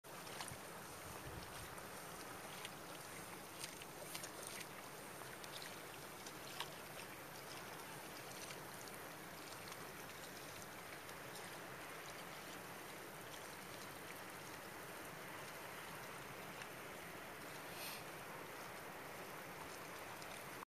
Padling 2ndre dag
Padling utenfor Risør andre dag jul.